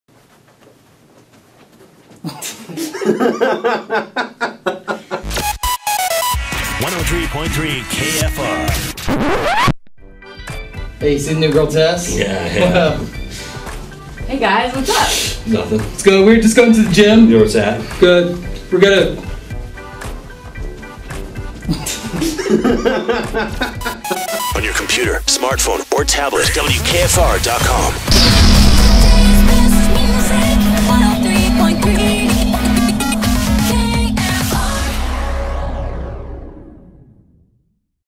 Morning Mayhem: How Chubby Guys Act Around Girls
Will and Dana have let themselves get a little too comfortable these days.

How will they act with a new girl in the building?